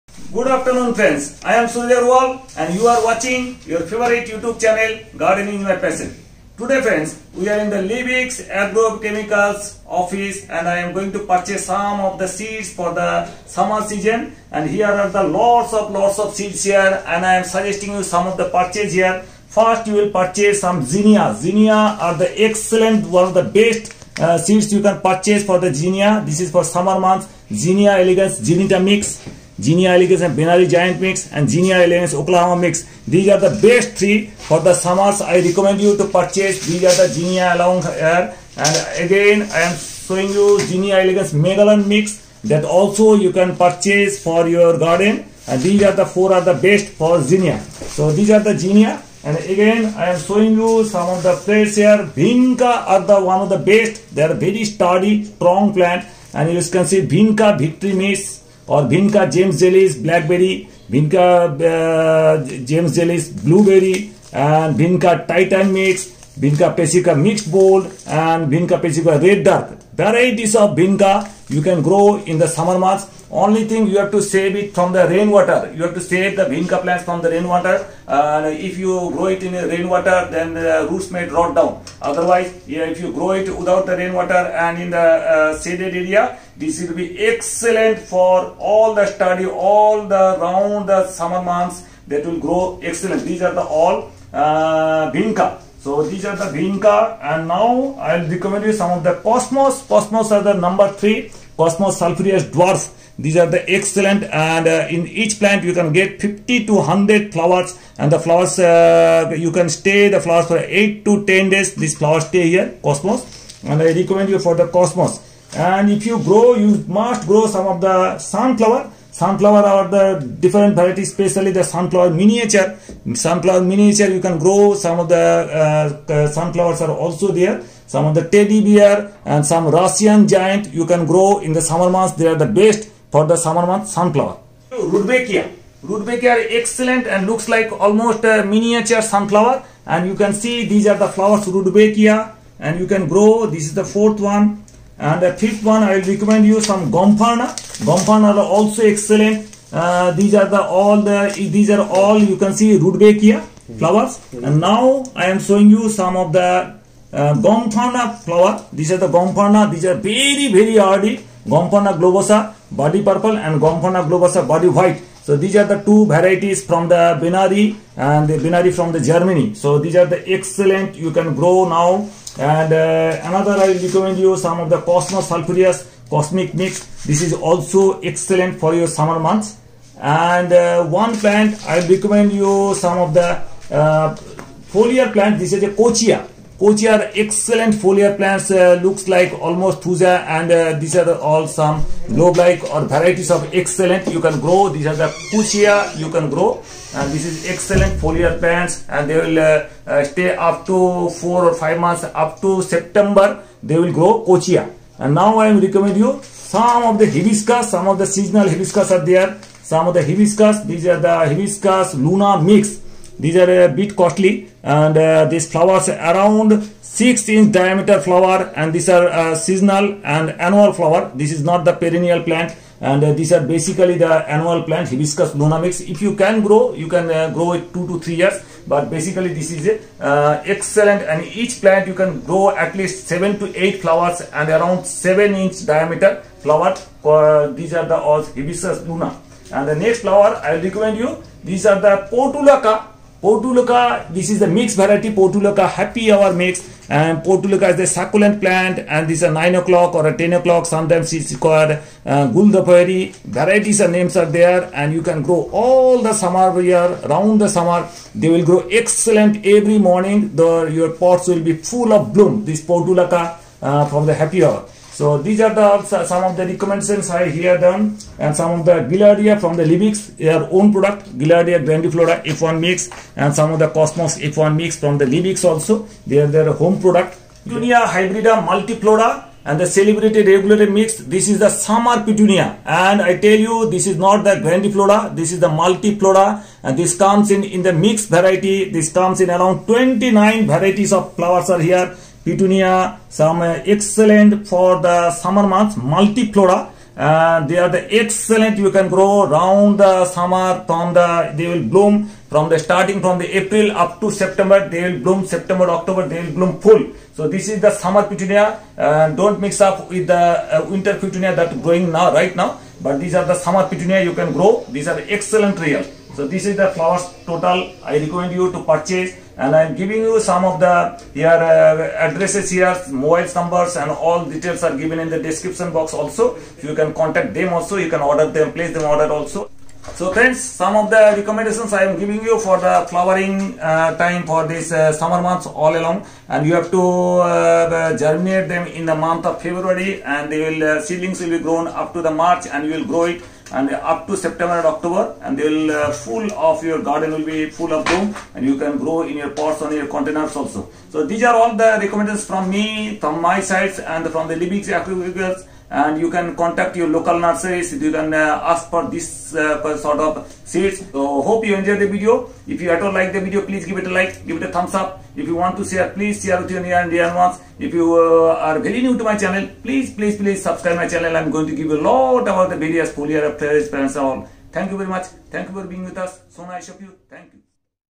Summer Flower Seeds You can Grow in the Month of February.
Here some Flowering Plants are recommended for Sowing seeds in the month of February so that your garen and Containers will be full of boom all along the Summer season.
For seeds you may contact your nearest Nursery or Seed store or Garden Stores.
Ajay Maiti
Leibigs Agrochemicals Pvt LTD
3A , Dharmadas Row
Kolkata - 700026
India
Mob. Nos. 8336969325